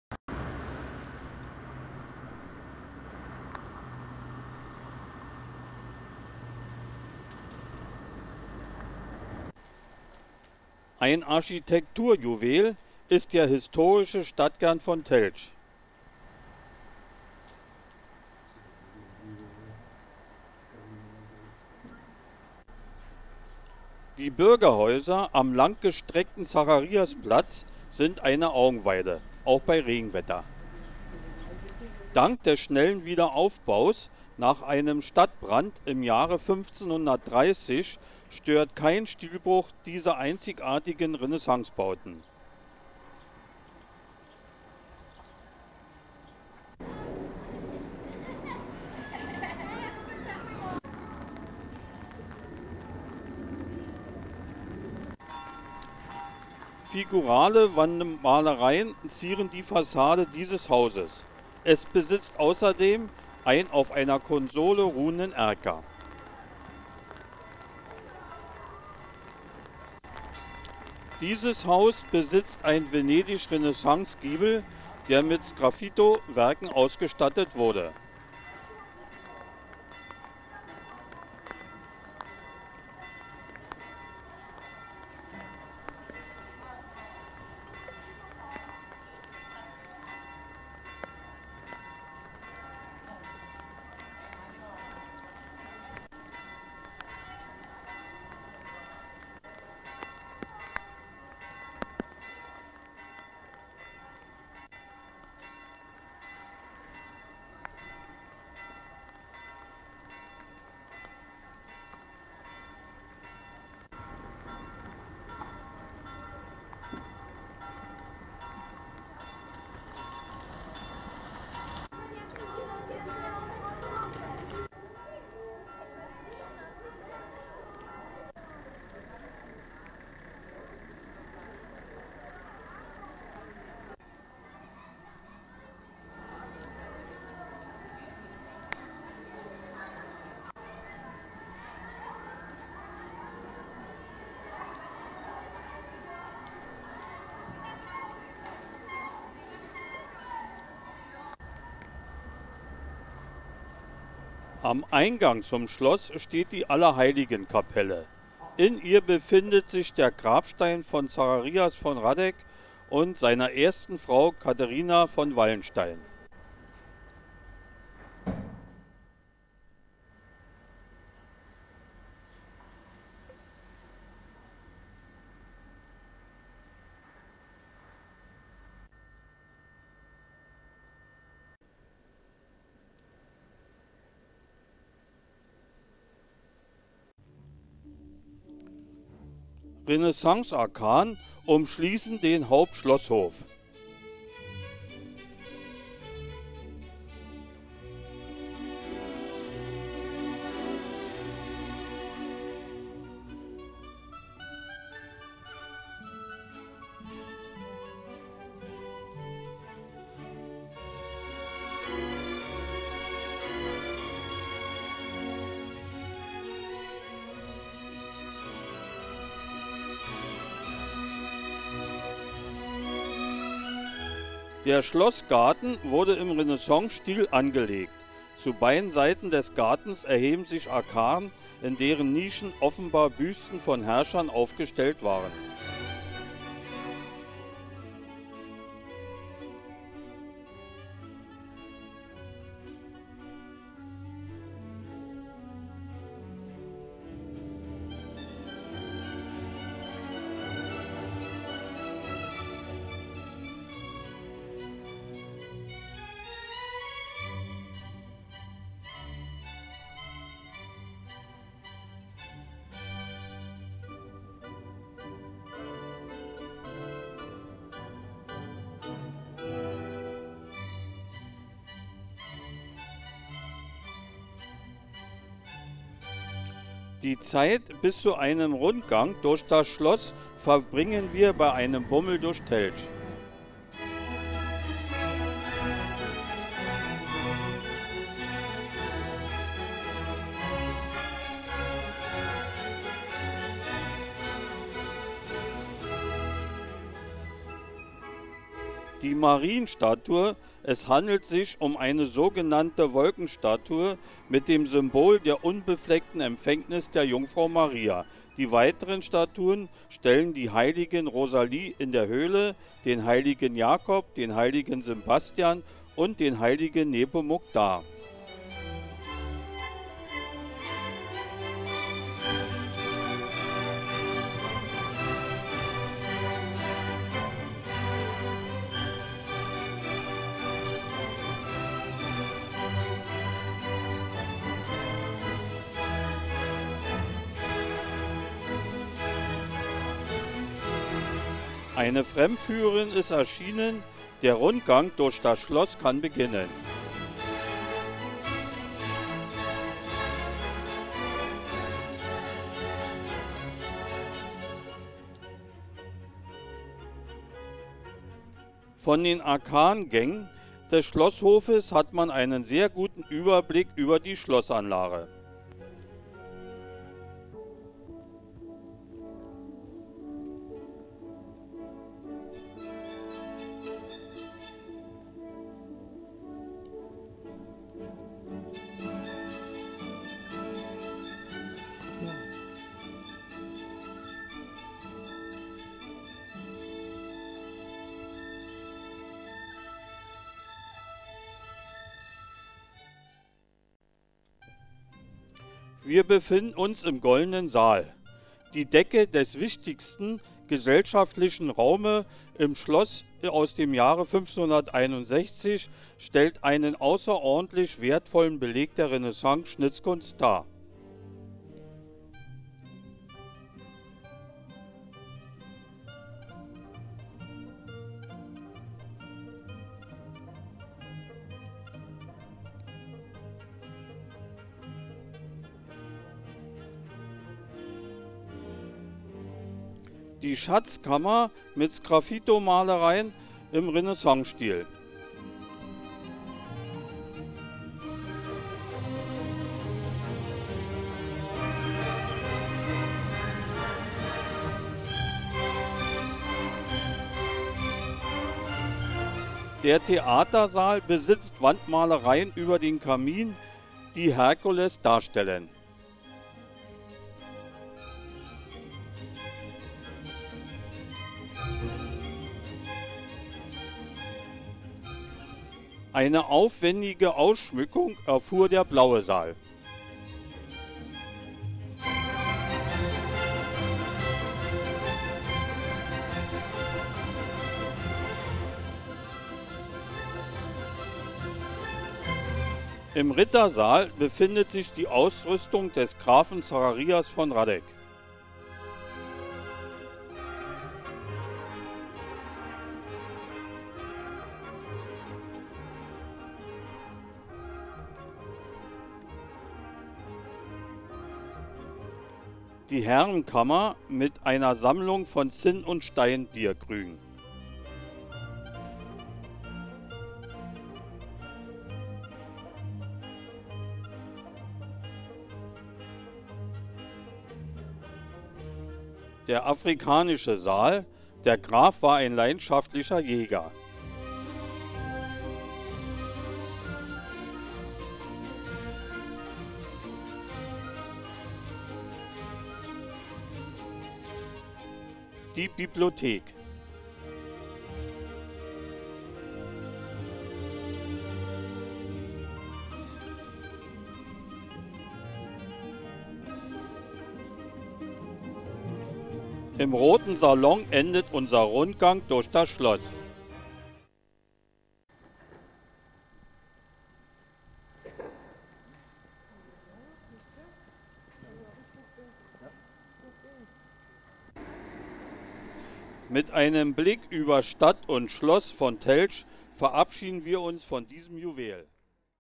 Teltsch
Teltsch ( Telc )
Ich möchte wetten , das es kaum einen schöneren Markplatz , als den in Telc gibt. Er ist sehr lang , von Toren abgeschlossen und ringsum von Laubengängen gesäumt. Jedes Haus hat einen hohen  Giebel mit schönen Konturen und mit Stuck und ist rosa ,blau oder weiß gestrichen. Die Fassaden dieser Häuser sollte man in Ruhe studieren. Mitten auf dem Markplatz ein bewundernswerter Brunnen.
Der Höhepunkt der Besichtigung von Telc ist das am Ende des Markplatzes befindliche Renaissance - Schloss.